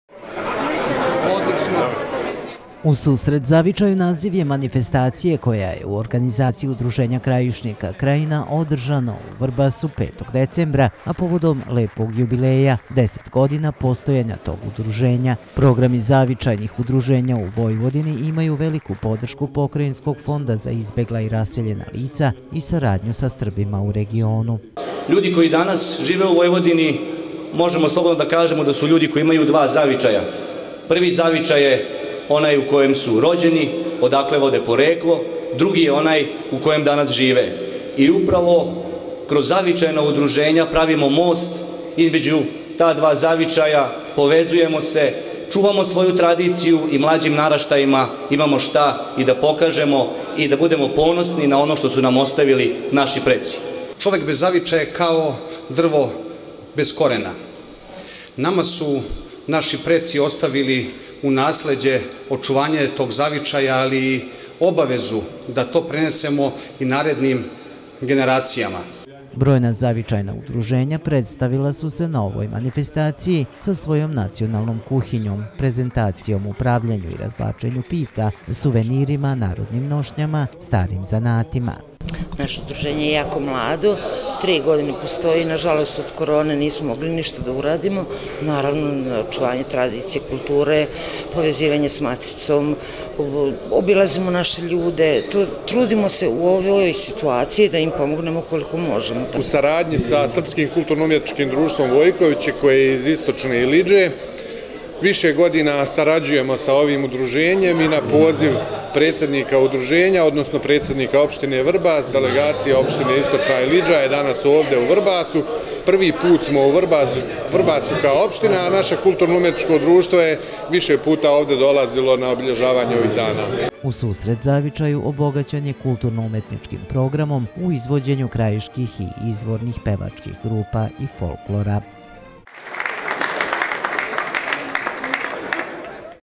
TV KANAL 9, NOVI SAD: U SUSRET ZAVIČAJU
Zavičajna okupljanja i manifestacije česta su Vojvodini gde su odavno postala deo kulturnog miljea Srba doseljenika I njihovih potomaka. Jedno od njih je održano u Vrbasu 5. decembra u organizaciji vrbaškog Udruženja Krajišnika Krajina.